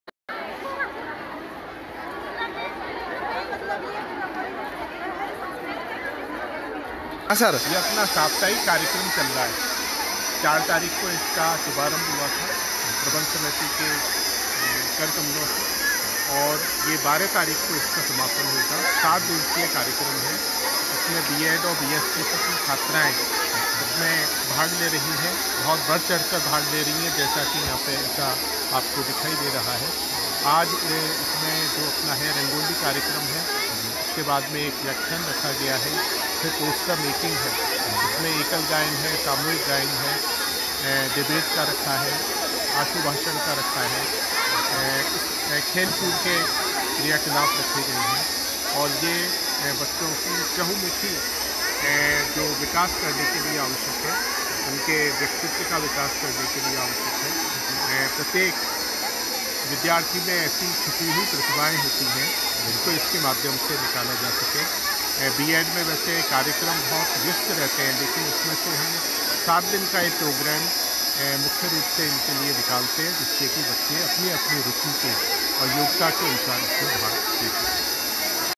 अग्रवाल महिला शिक्षण प्रशिक्षण महाविधालय गंगापुर सिटी में साहित्यिक, सांस्कृतिक एवं खेलकूद प्रतियोगि
अग्रवाल महिला शिक्षण प्रशिक्षण महाविधालय गंगापुर सिटी में साहित्यिक, सांस्कृतिक एवं खेलकूद प्रतियोगिता आयोजित की गई